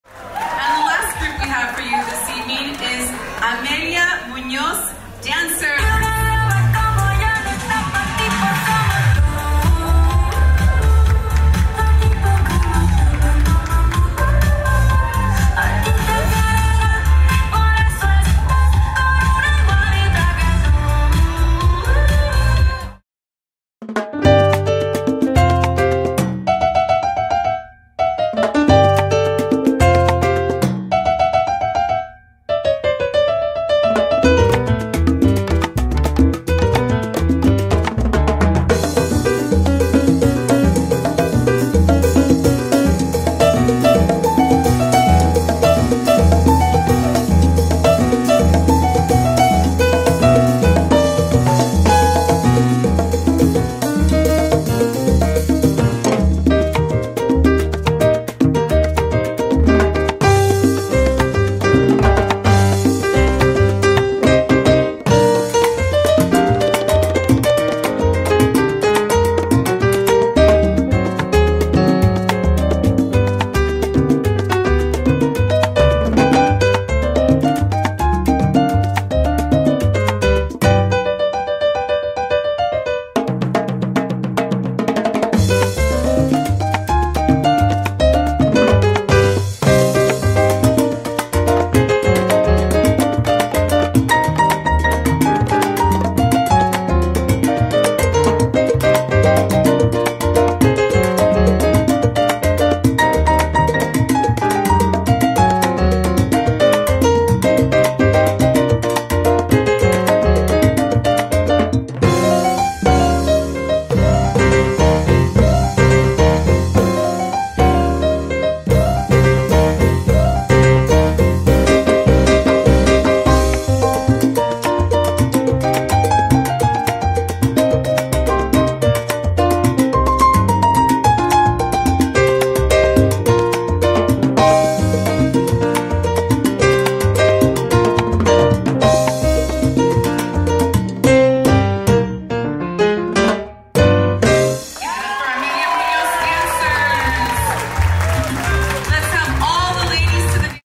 Amelia Muñoz Dancers
Ladies Night Out
Thursday, March 9th at Stevens Steak and Seafood House Restaurant
Performance by Amelia Muñoz Dancers